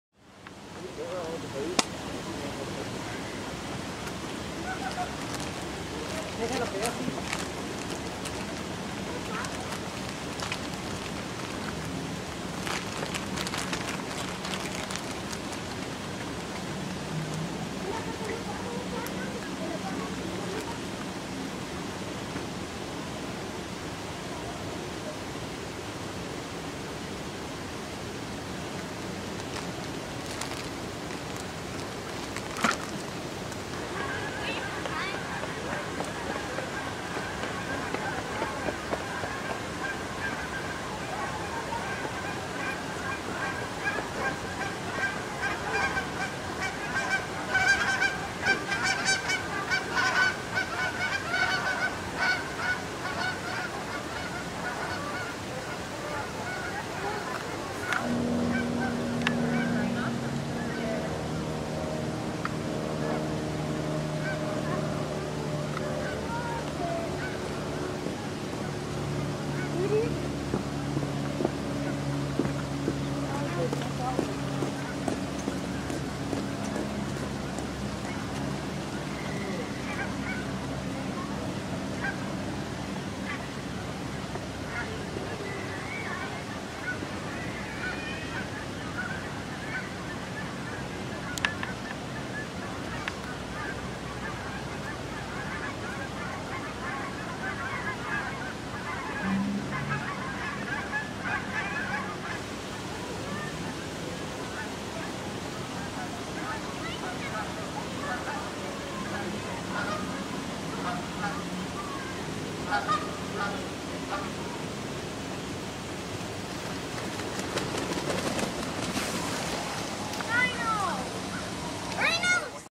Rhino at the Denver Zoo
Rhinoceros at the Denver Zoo. I think his name is Bandhu.